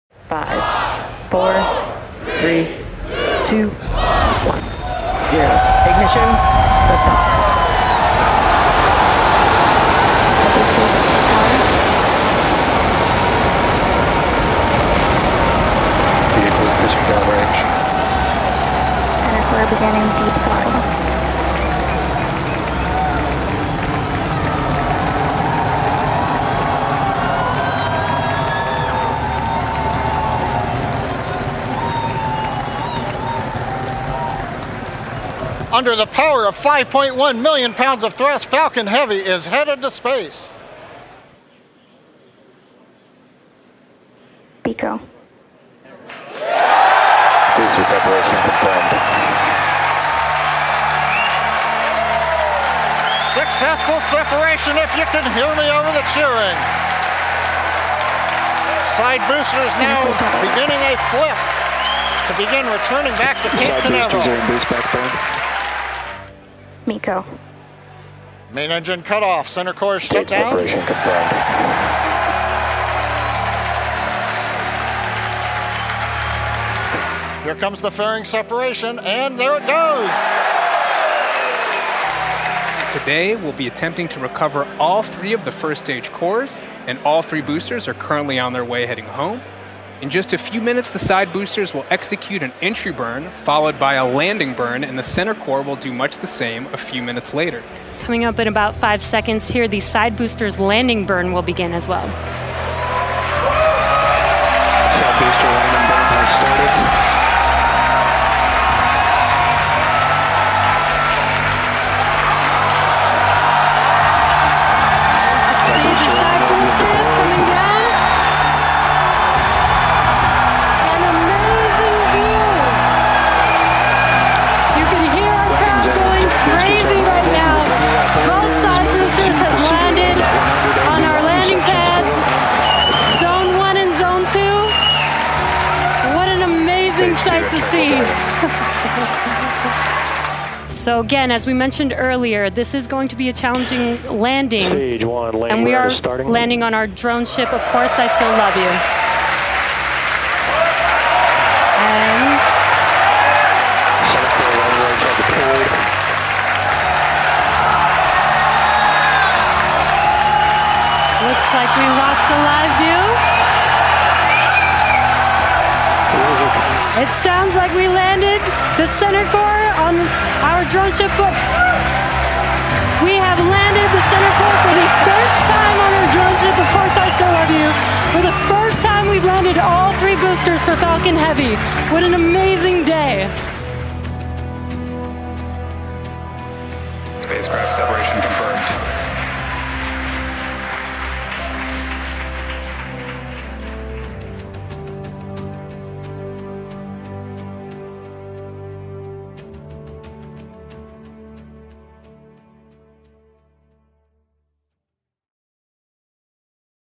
Falcon Heavy #2 (SpaceX) Launch & Landing
On April 11, 2019, SpaceX successfully launched their second Falcon Heavy and landed all three boosters safely.

Other than just condensing the footage, I did make a few noticeable edits near the end when the video feed lost connection; however, they're only to assist the pacing of the video and in no way change the meaning of the content.

0:00 - Launch
0:42 - Booster Separation
1:05 - MECO
1:18 - Fairing Separation
1:43 - Side Boosters Landing
2:52 - Center Core on Drone Ship
3:03 - Spacecraft (Arabsat-6A) Separation